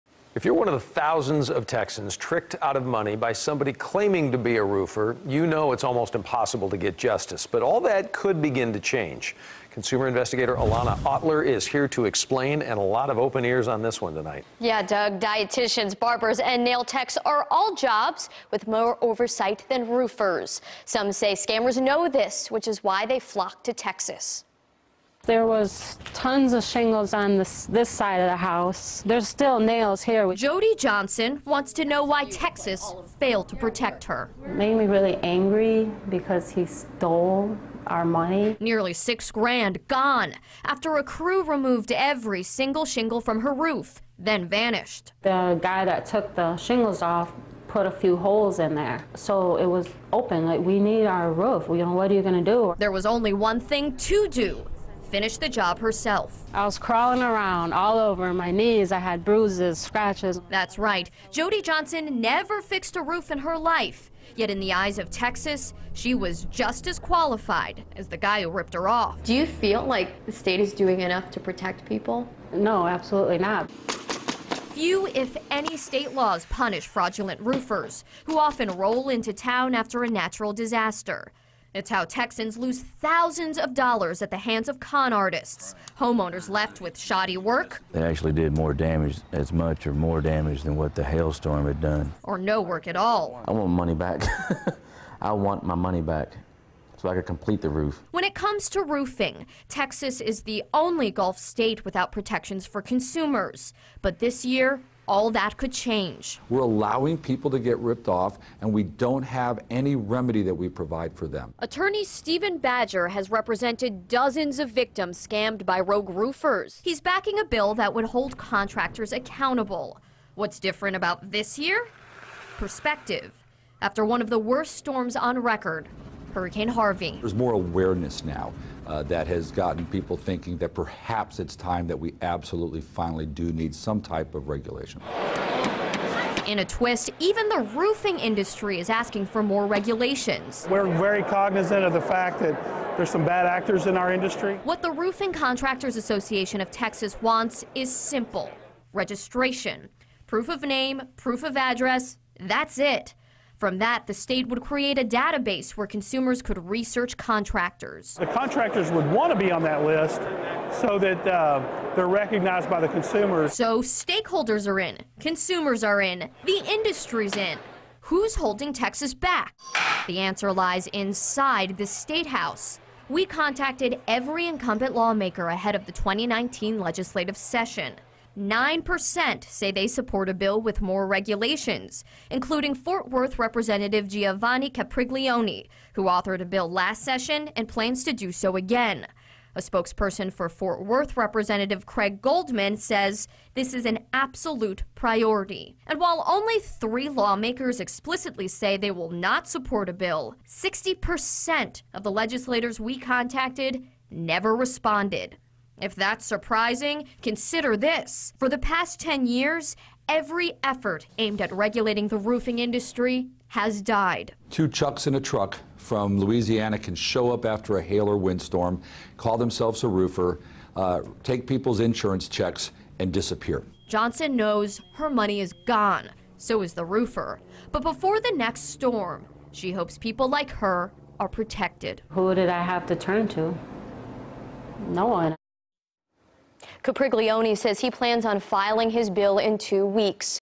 Victims Of Fraudulent Roofers Could Possibly See More Protections
Dietitians, barbers and manicurists are all jobs with more oversight than roofers.